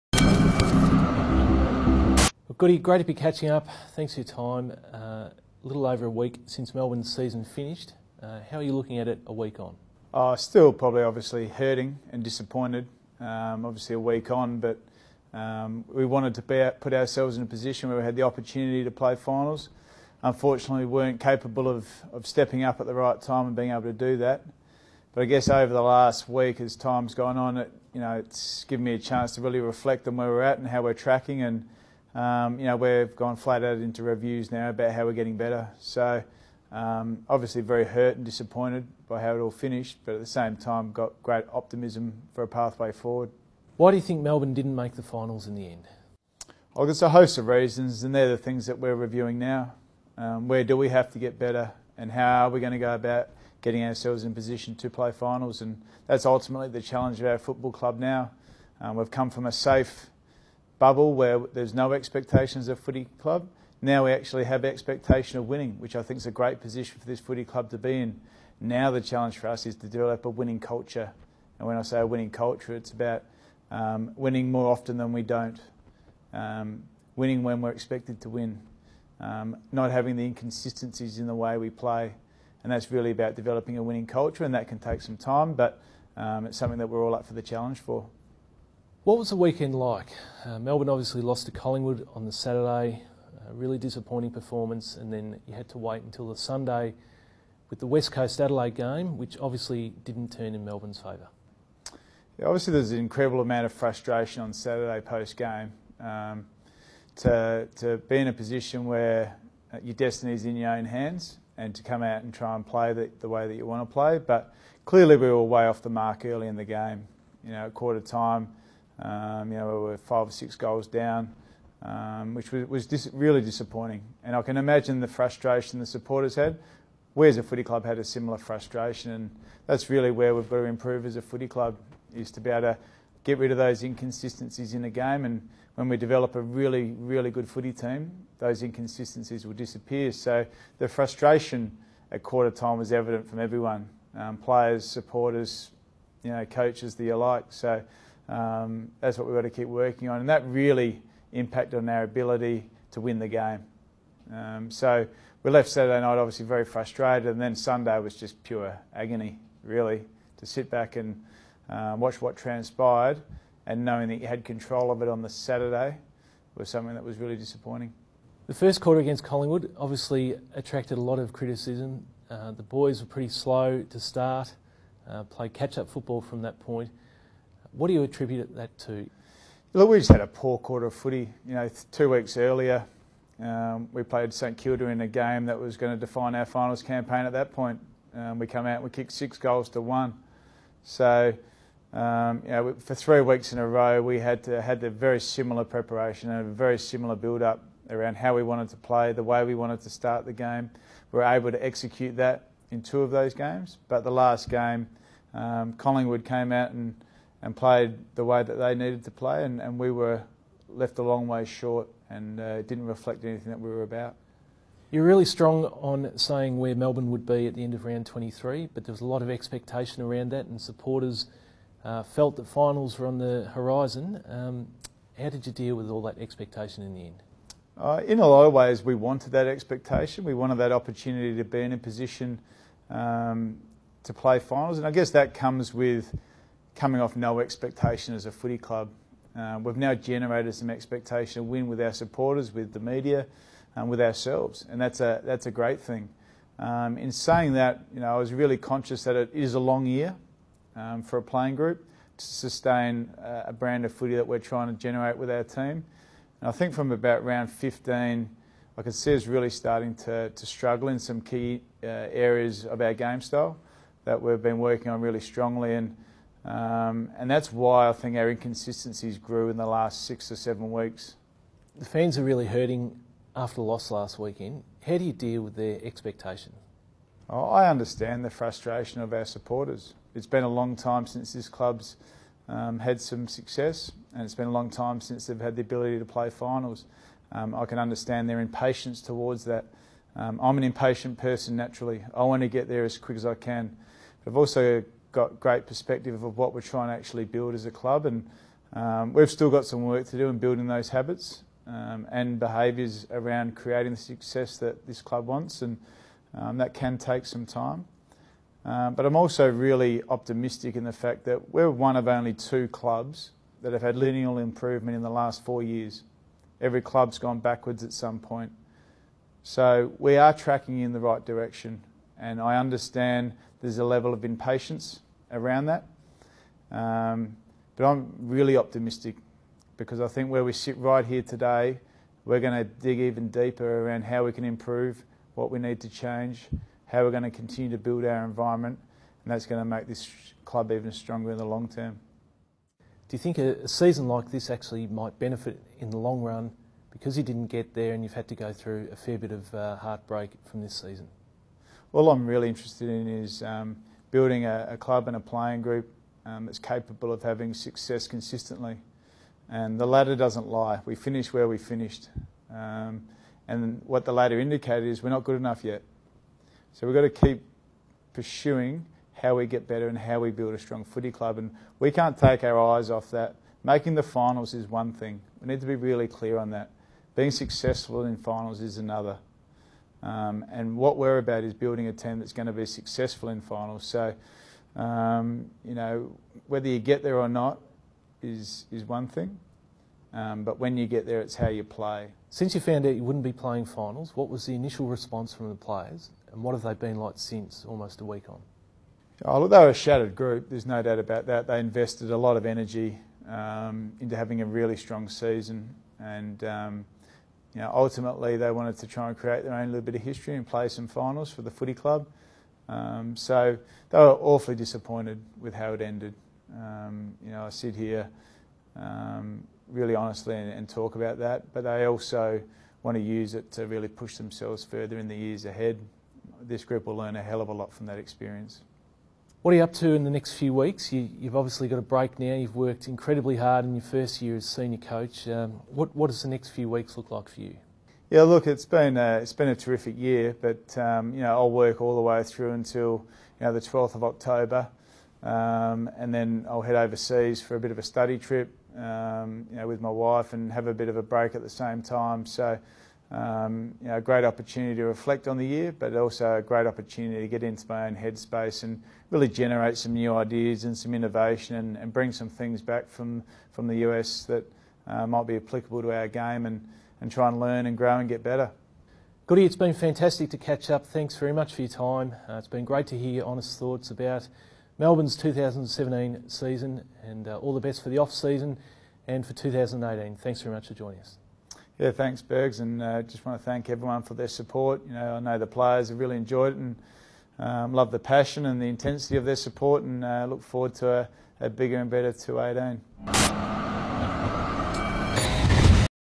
Missed Opportunity: Simon Goodwin
Matt Burgan sits down with Simon Goodwin to review the 2017 AFL season